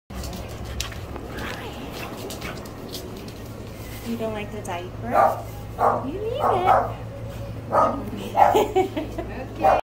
So Excited, He Pee'd His Pants
Gronk loves to mark when we're in public. When I picked him up, the groomer had a diaper on him which he absolutely disliked. 😆